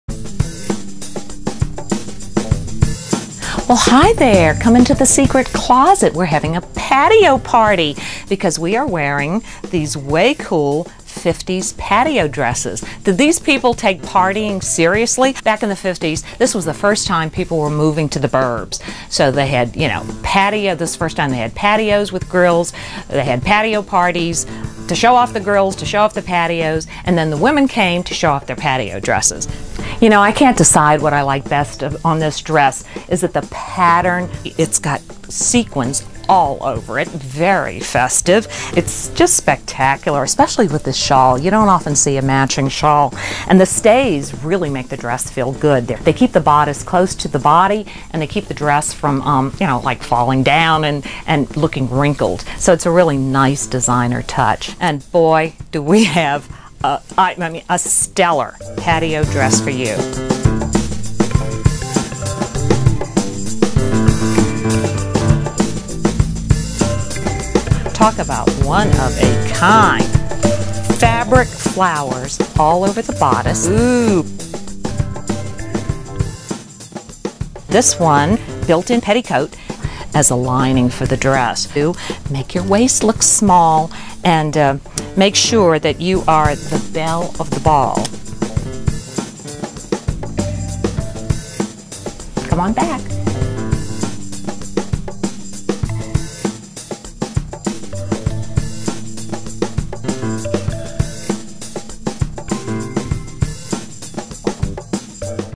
Get the Look: Secret Closet Vintage--Incredible Patio Dresses!
SecretClosetVintage--Talk about channeling Doris Day and Sandra Dee! Bar-b-que and highballs on the “patio” was oh, so chic in the 1950’s.  And it’s no wonder with the gals wearing these amazing patio dresses! We adore this striking two-piece floral patterned dress in shades of rose, coral, red and green. The bust line cradles a garden of fabric roses bordered with an olive velvet ribbon that continues as straps on the shoulders and ends in a flirty bow in the back.  The full skirt with built-in petticoat invites you to swish through the party – breaking hearts along the way!

The label on our next 1950’s patio dress is that of the legendary Mexican fashion designer Tachi Castillo. It features dramatic Mayan figures in blue. Hand-sewn navy sequins embellish the dress both front and back. Narrow shoulder straps make this unique dress look almost strapless. There is also a matching shawl! This patio dress is simply gorgeous! Gracias, Tachi!

We want to hear about your own vintage fashion passion!
Send us your photos and stories or any inquires